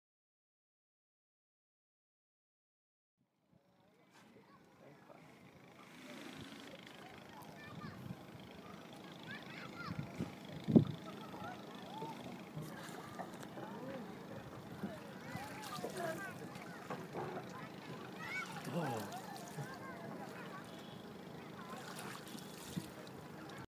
05 Irrawady Dolphins
These dolphins are in their last 100s so we made sure we went to see them. They are fresh water dolphins living in the Mekong River. They  have a small population in Laos and Cambodia. We had at least 20 sightings during our 1 hour boat ride. The locals were very well trained and kept their distance, however, the dolphins will occasionally come very close to us!